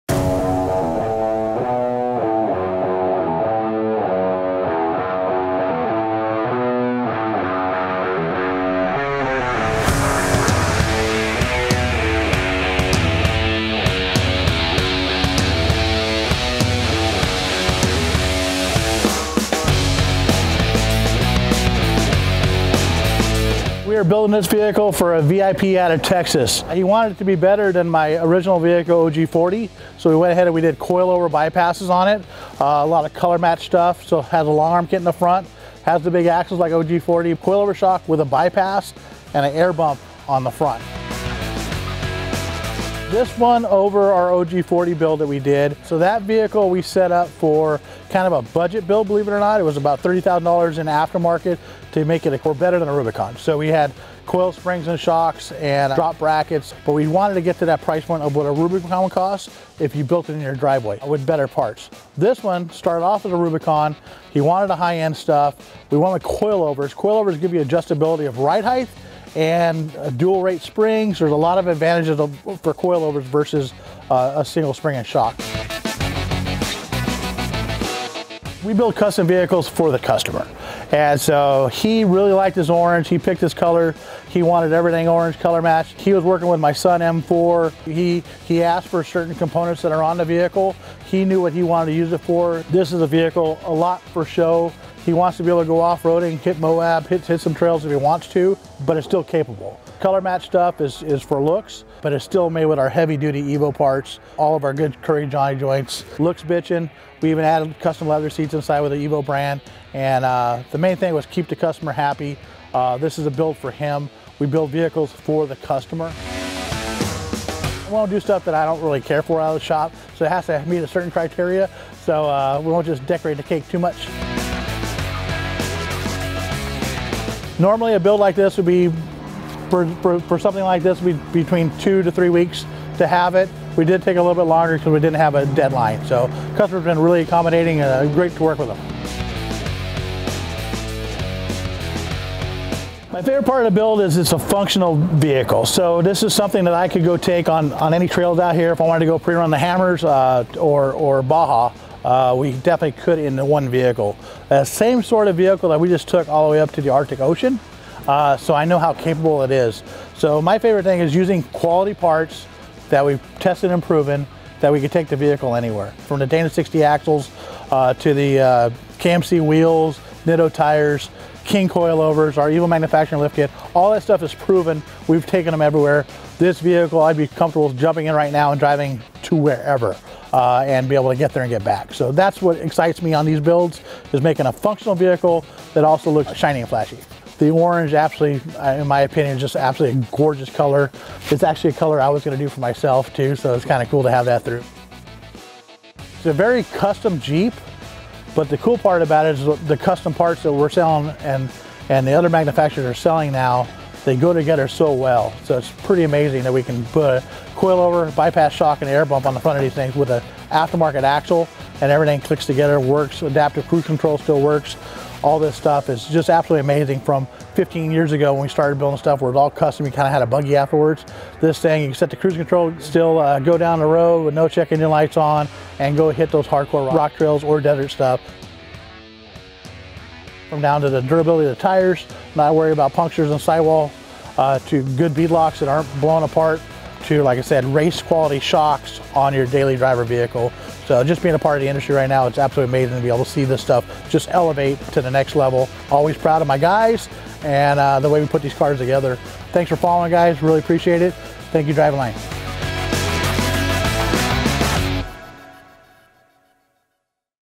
Meet Bevo: The Texas Longhorns Inspired Jeep Gladiator
Brad Beldon's 2020 Jeep Gladiator JT Rubicon all started with the paint color. He got in contact with Mel Wade and the guys at Off Road Evolution to build his new Gladiator that he could not only show-off, but take wheeling wherever he chose. Brad also wanted to honor the University of Texas at Austin and their official Longhorn mascot, Bevo.

Complete with a full set of front and rear KING coilovers long travel custom suspension, 40-inch Nitto Tire Trail Grapplers, and outfitted with a full buffet of Off Road Evolution’s rugged Jeep parts. This Gladiator was build tough and Longhorn proud!